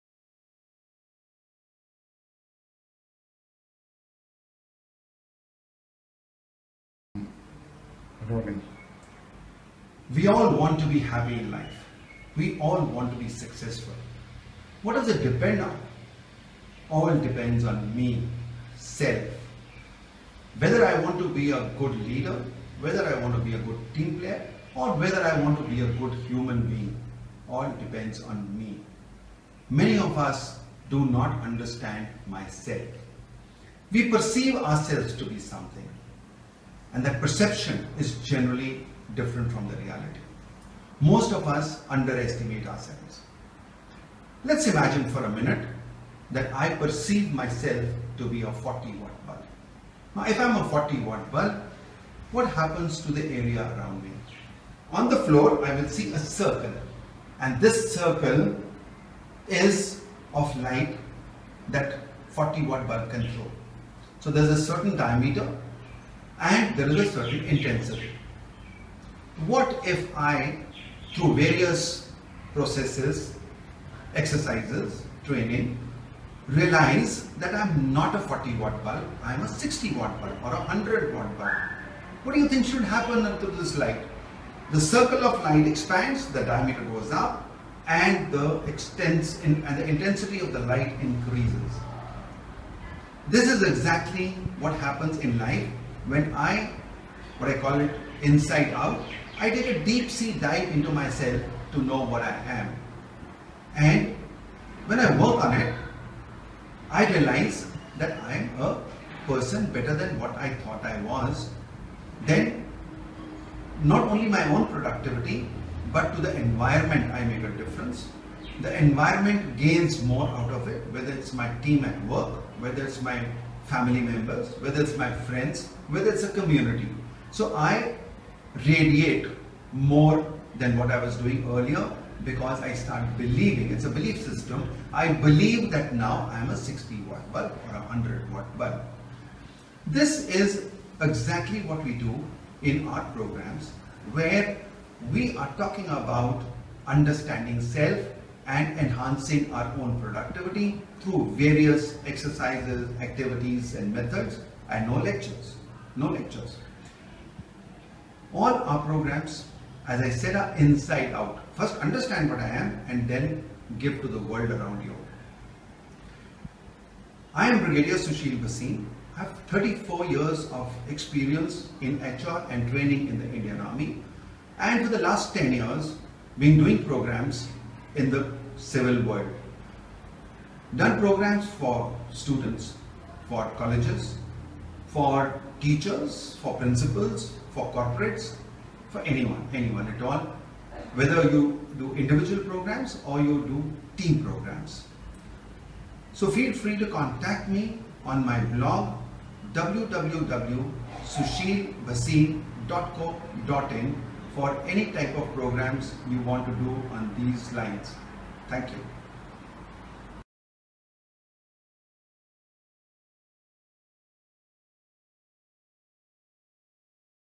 Deep Sea Dive
Brig Sushil Bhasin conducts program on self development. He makes you take a look into yourself and see if you know yourself well enough